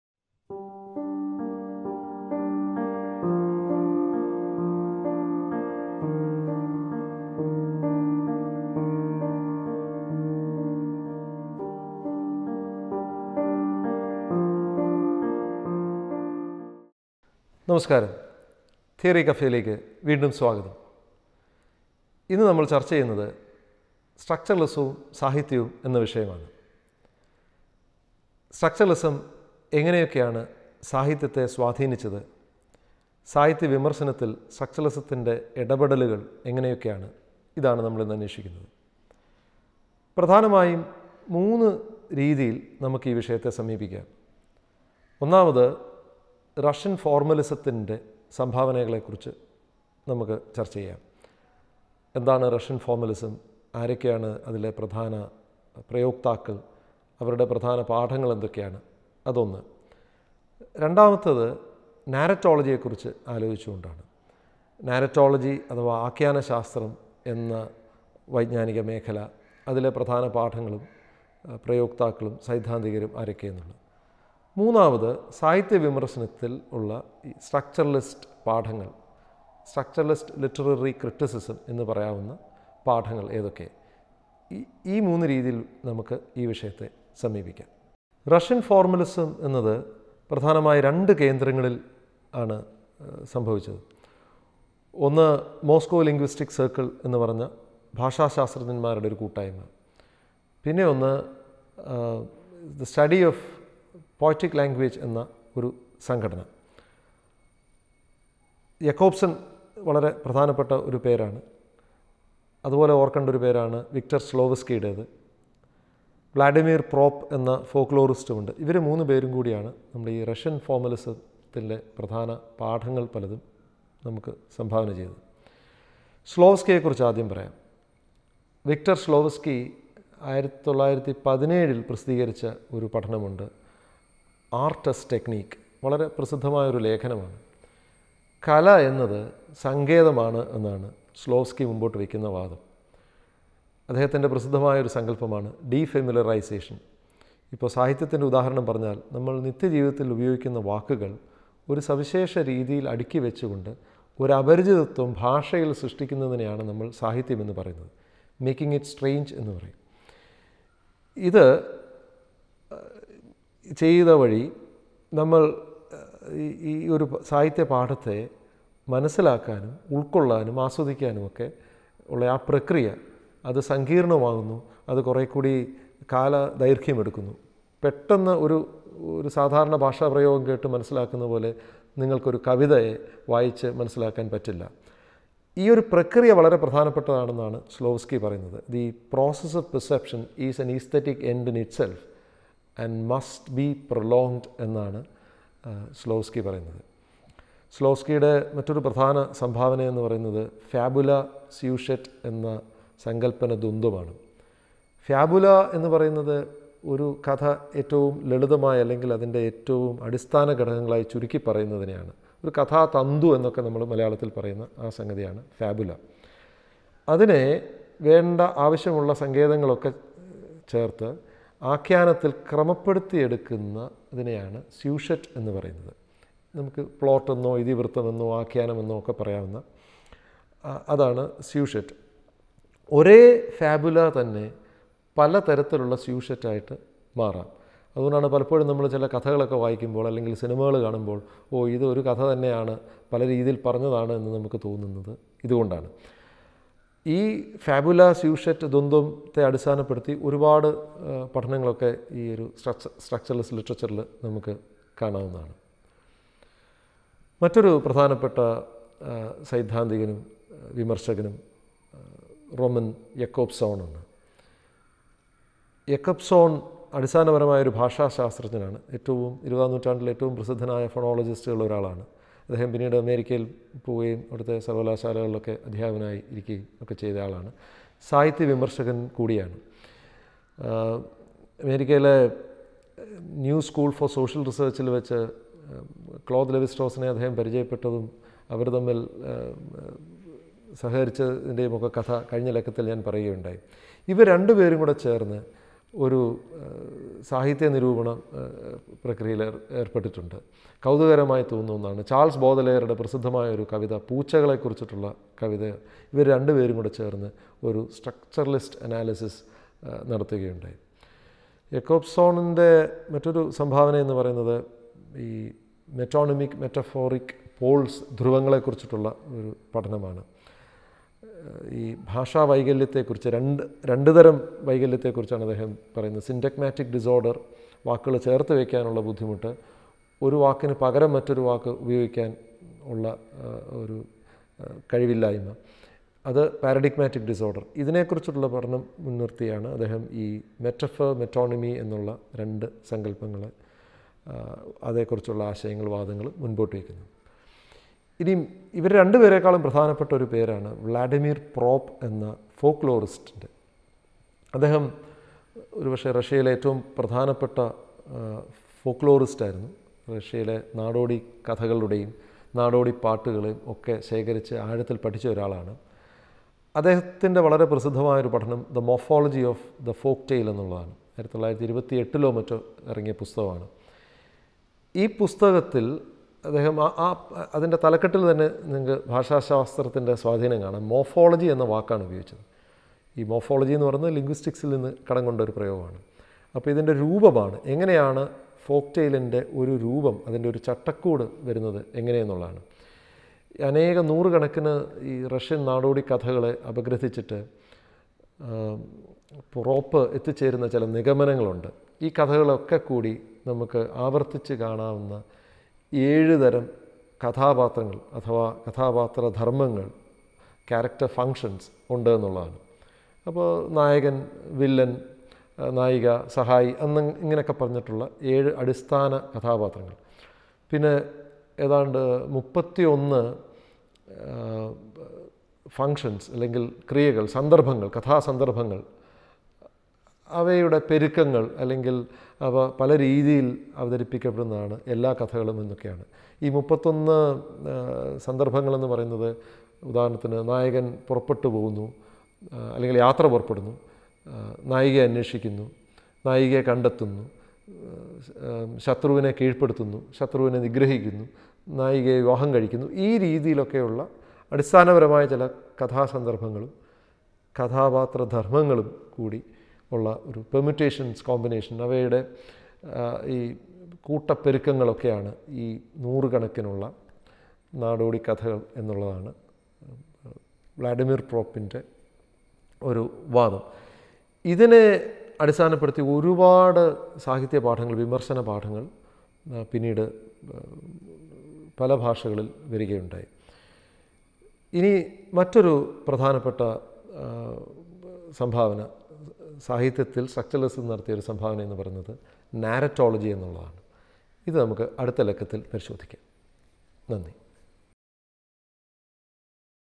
Theory Café | Episode 5 | Structuralism and Russian Formalism | Josy Joseph | SB College
BERCHMANS TV